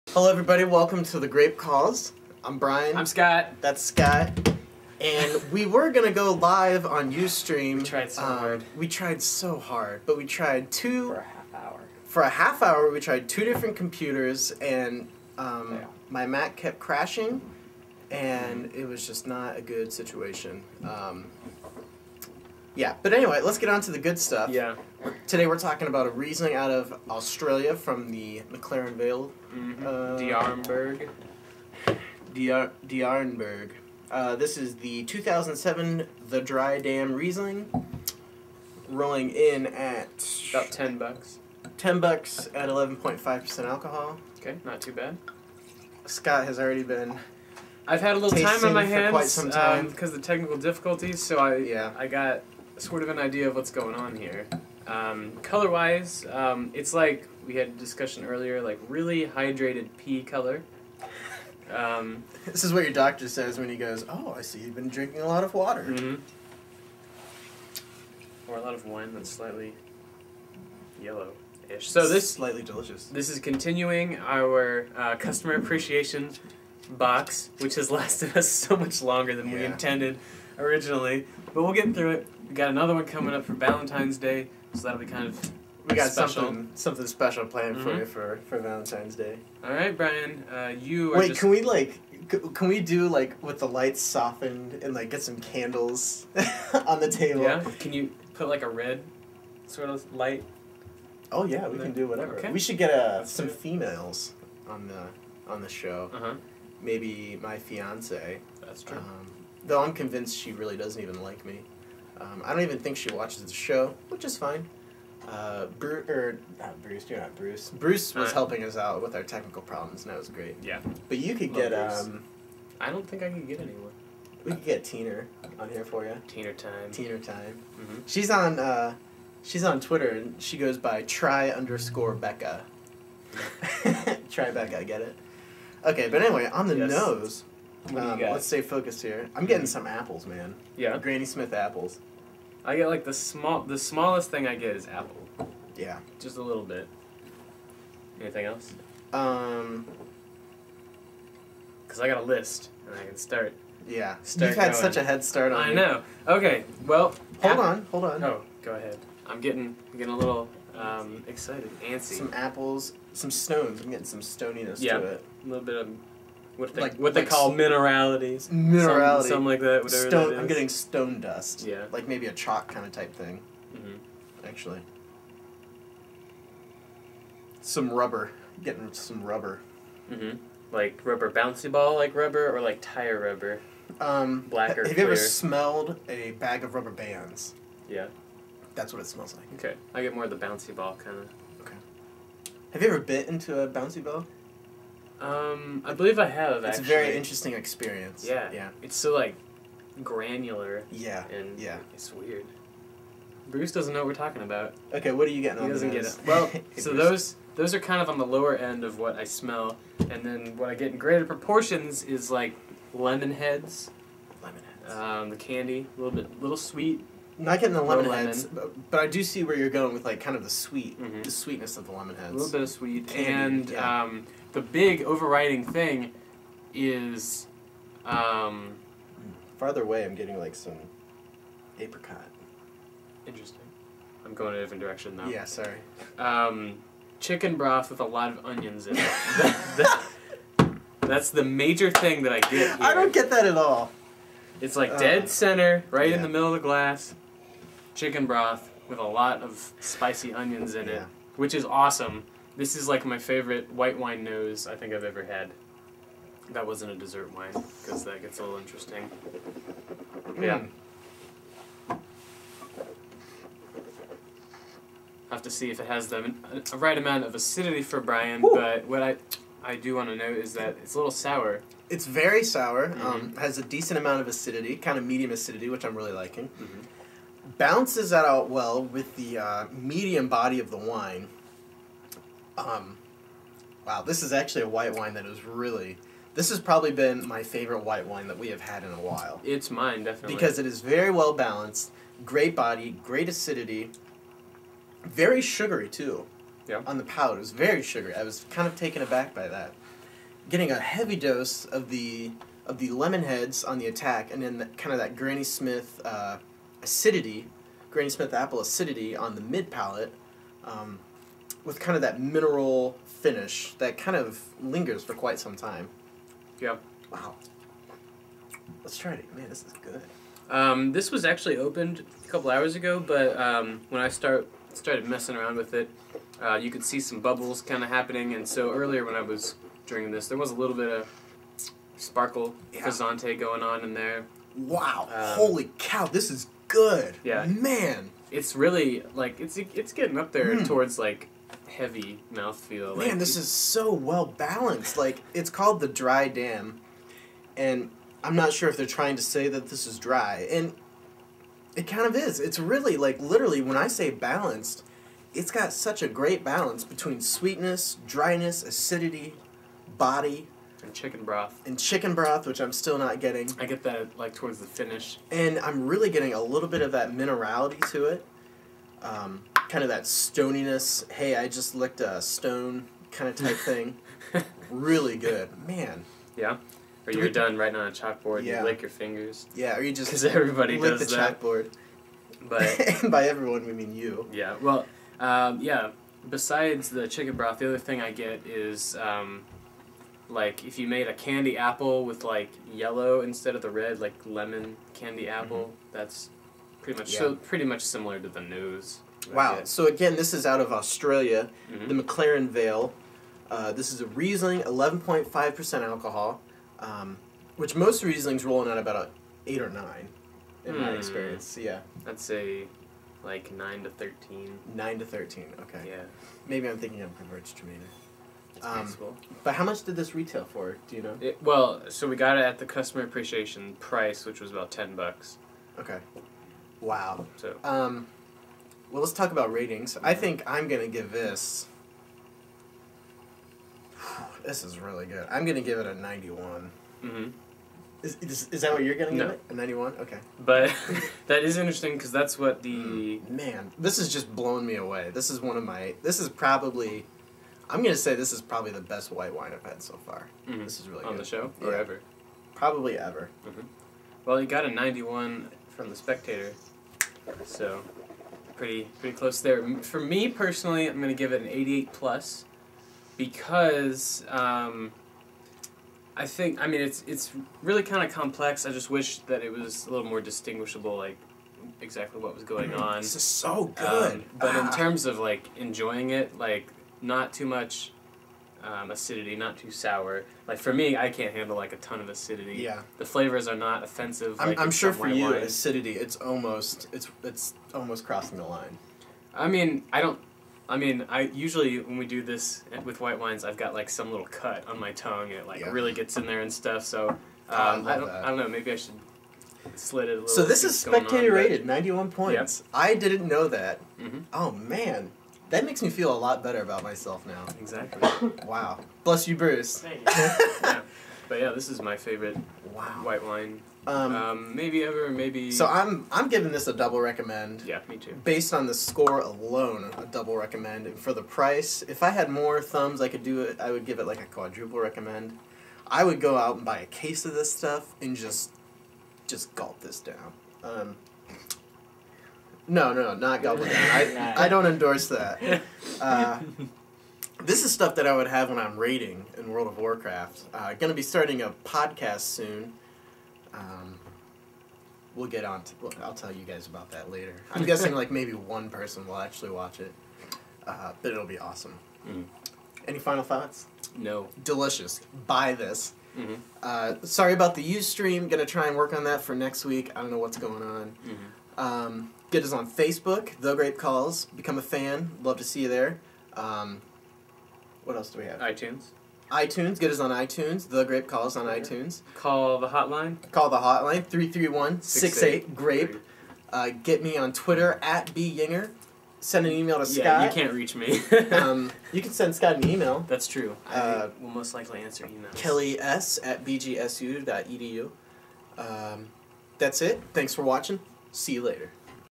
Episode 15 - D'Arenberg The Dry Dam Riesling 2008
Scott and Brian taste one heck of a white wine value.